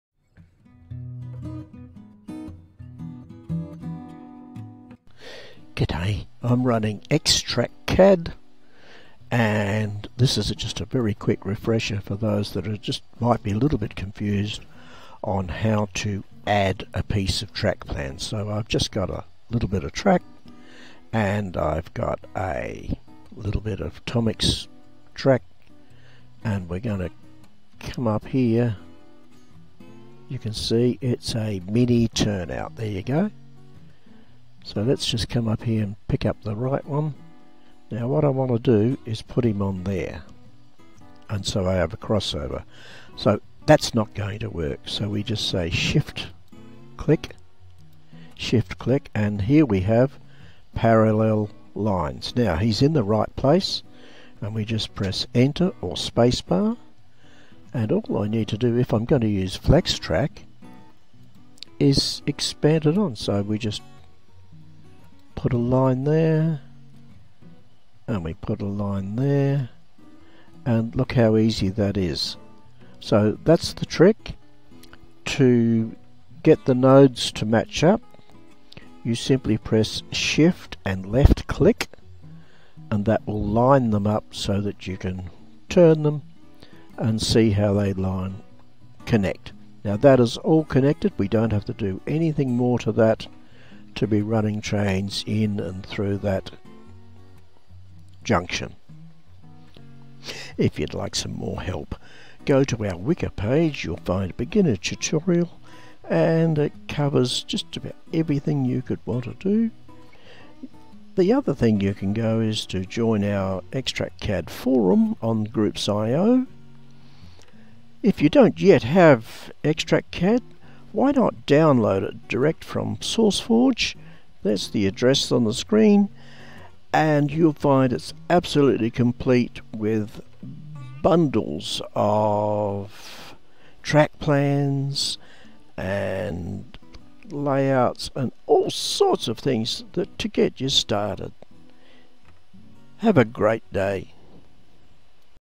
XTrkCAD: Attaching and rotating track components
Some have been having problems attaching track components within XTrkCAD when the added component doesn't line up properly - that is - the track piece tries to connect to the wrong Node.
I demonstrate that using Shift-Left Click will rotate the additional track piece through the available Nodes until the correct Node is connected. Then simply press the Space or Enter key and it will be locked into place.
If anybody wants to know more about XTrkCAD drop into the XTrkCAD
Or sign up to the Forum on Groups.io and you will find all the help you could look for from the Members.